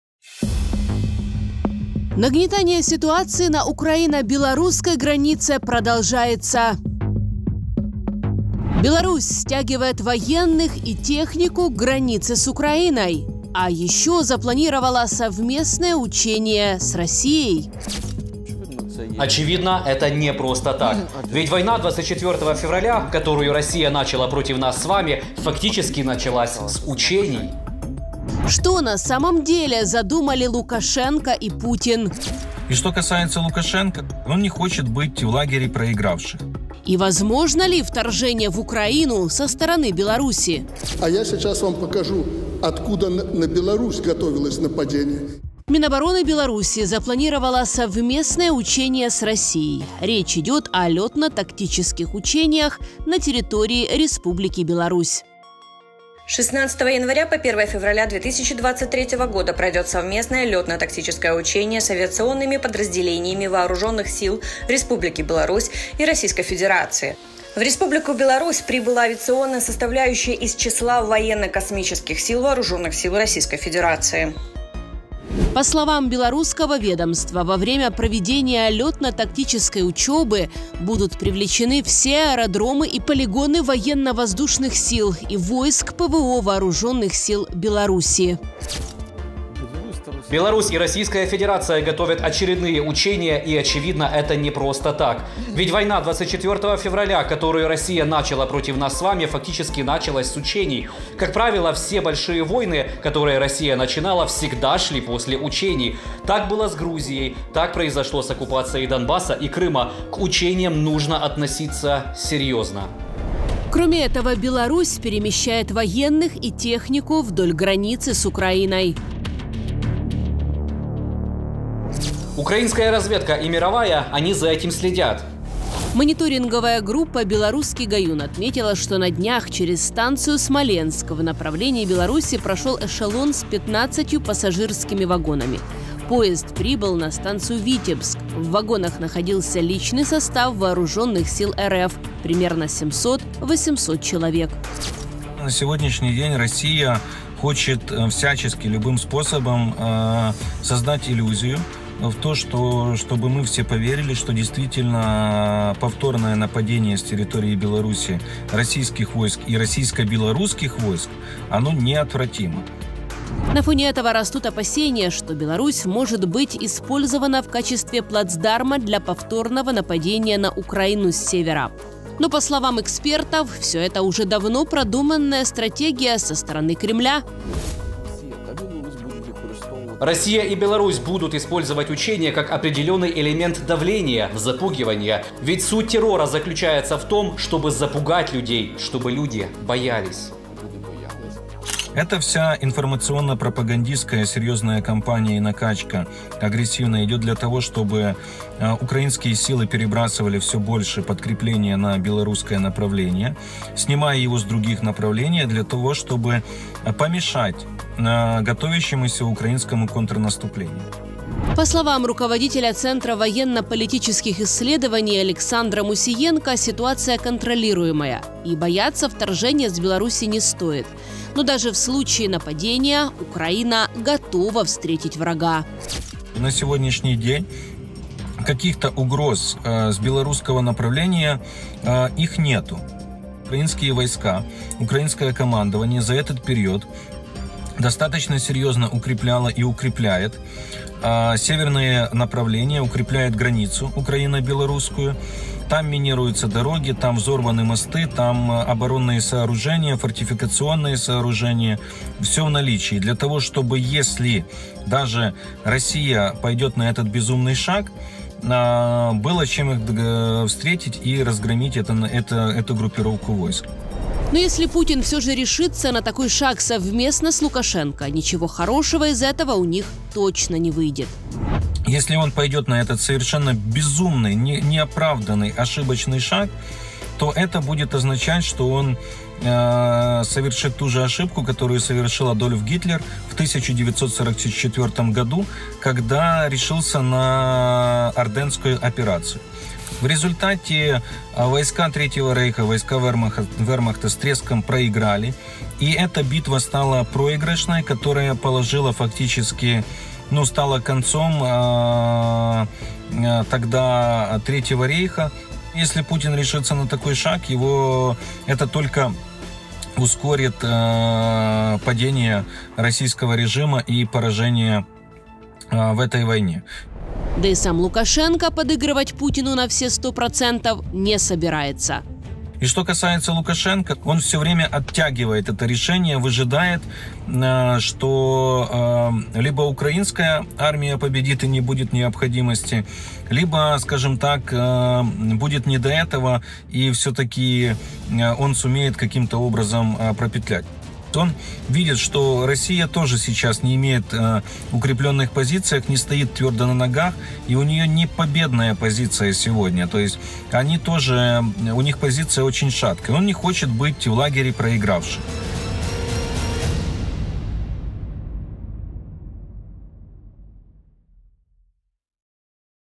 🤔Беларусь стягивает военных и технику к границе с Украиной
Возможно ли вторжение в Украину со стороны Беларуси? Что на самом деле задумали Лукашенко и путин? Беларусь стягивает военных и технику к границе с Украиной. А ещё запланировала совместные учения с РФ. Чем все это может обернуться для Украины, смотрите в видео.